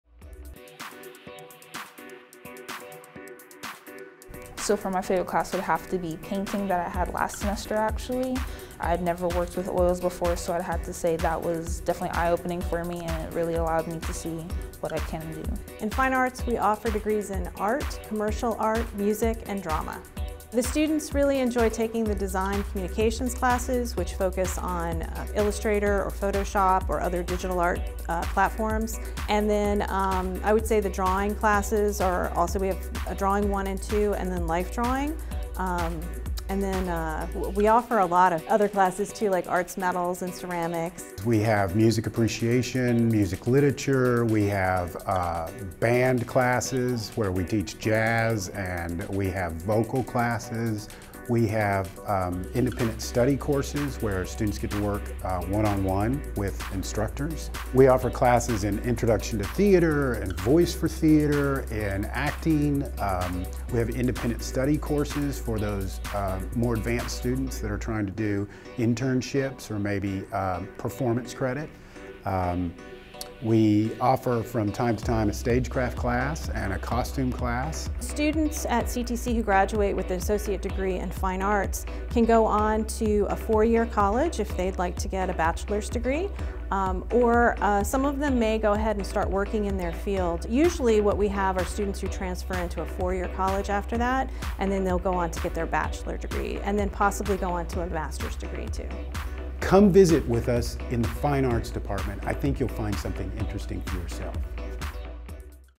Explore programs- Fine Arts
The Fine Arts Department at Central Texas College offers Associates Degrees in art, commercial art, music, and drama. Here is a look into classes that Central Texas College offers within these fields.